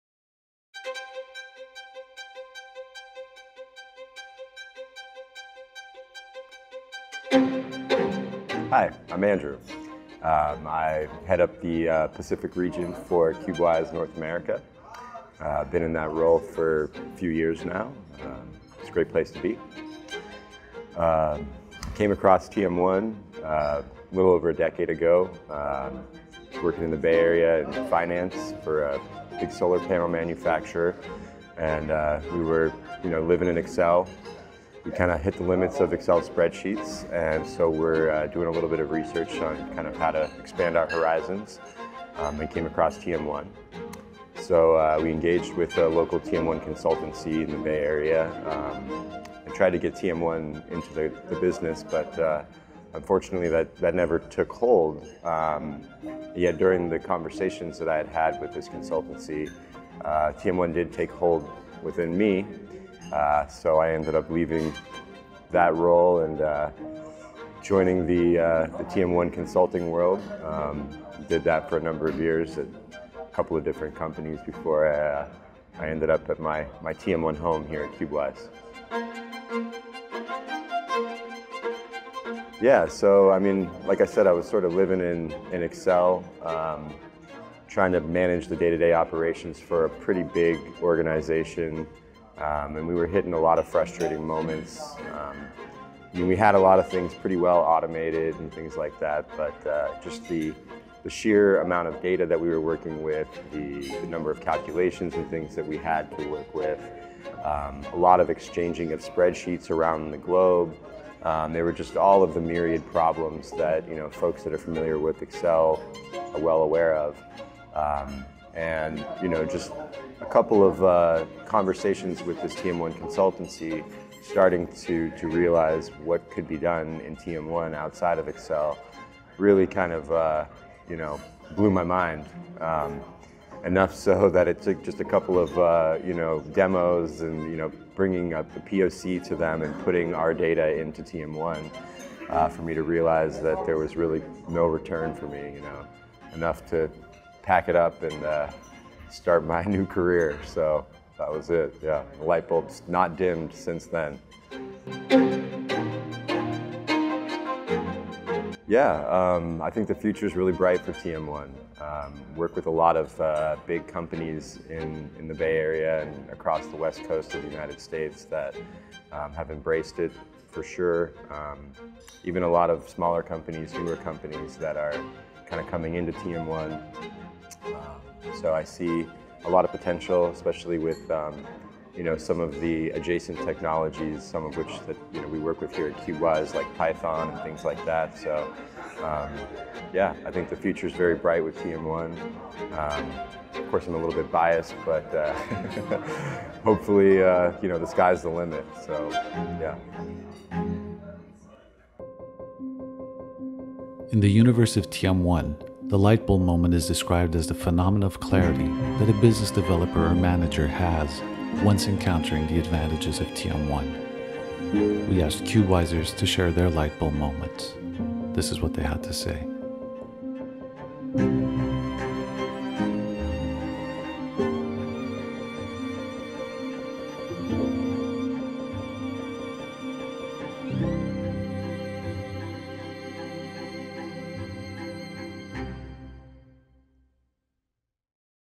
Andrew Leighton - The Lightbulb files
00:00 - Intro
00:08 - What is your background ?
01:21 - What was your lightbulb moment ?
02:37 - What is the future of TM1 ?
03:22 - Outro

Meet Andrew, Cubewise North America's Regional Director. He went from working with the biggest light source in the solar system, to having his lightbulb moment with TM1. Originally hailing from the Office of Finance in a big solar panel manufacturer, to becoming a TM1 adept and advocate.